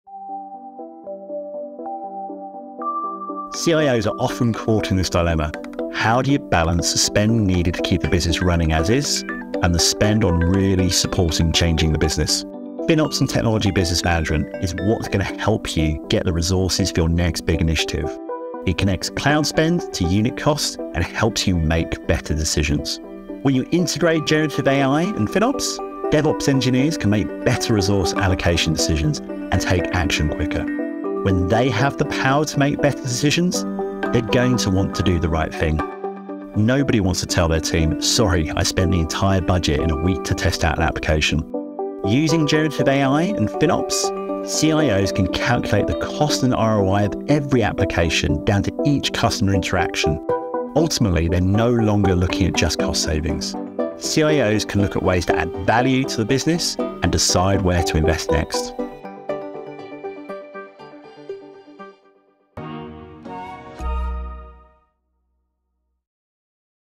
Rethink IT spend with AI
When it comes to IT, where do you invest? Declan Coombs, Cloud & Technology Advisory Partner at IBM, shows how FinOps and AI can empower teams to make better decisions and add value to the business.